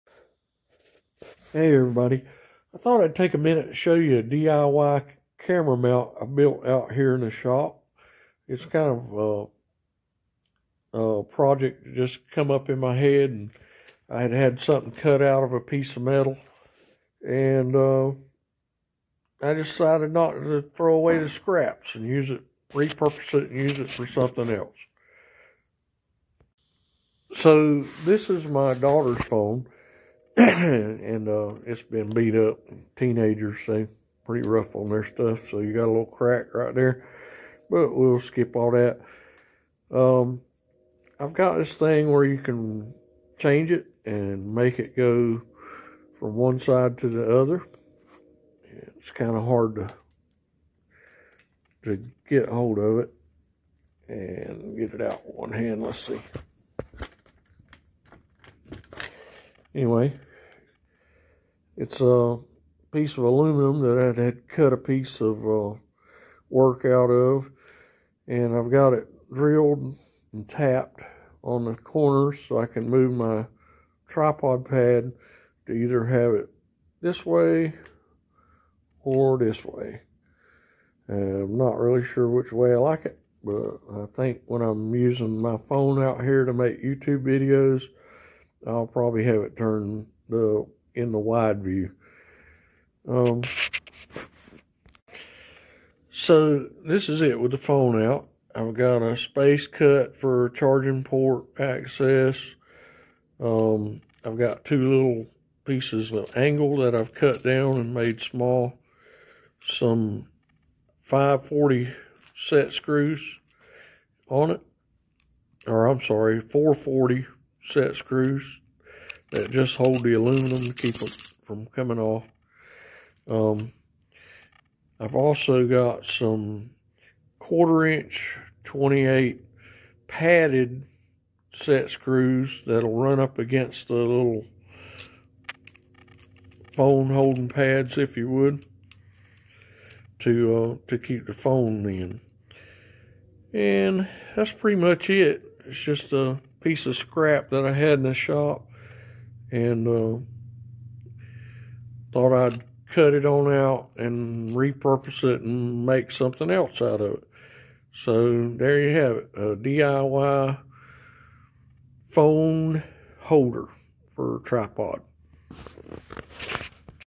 DIY Tripod Phone Mount
A piece of scrap turned into a useful tool...
sometimes you just see things can become more than they seem...  one man's junk, another man's treasure...